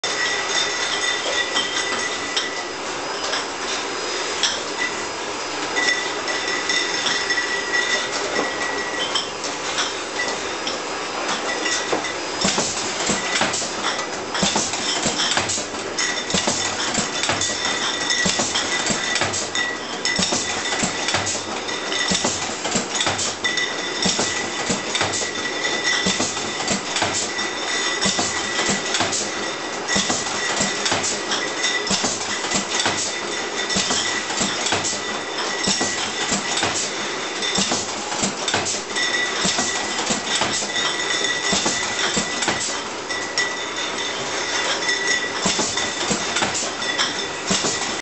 Embouteillage du cidre du Père Mahieu
Mise en bouteille du cidre Cotentin fabriqué à la Cidrerie Le Père Mahieu à Bricqueboscq dans la Manche en Normandie.